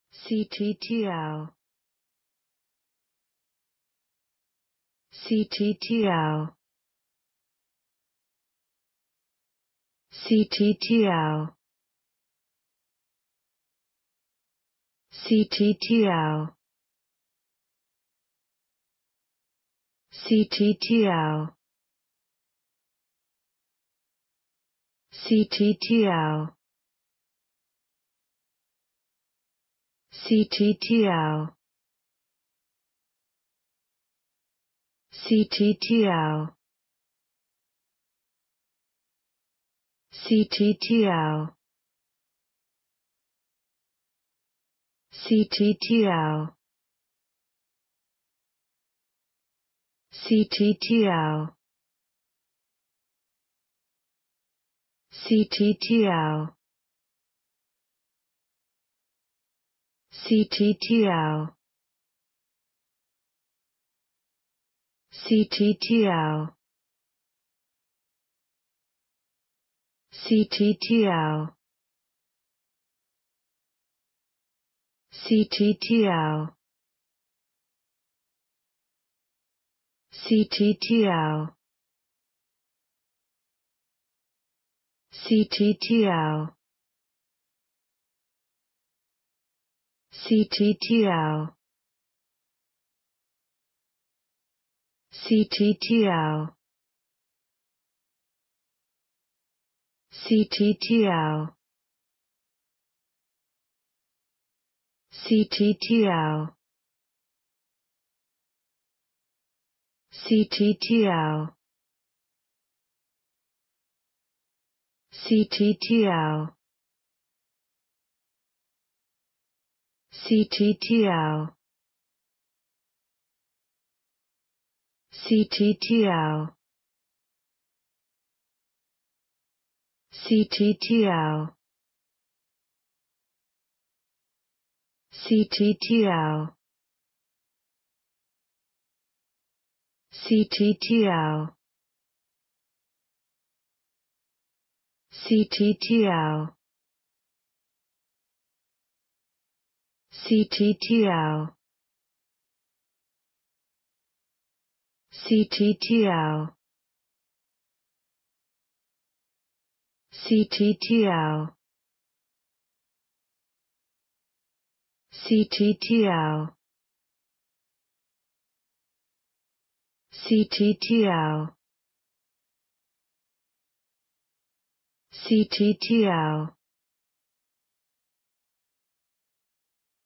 how to pronounce the english word CTTL.
Follow me!Learn from me how to pronounce the english word CTTL.This video shows you how to pronounce the English word  CTTL .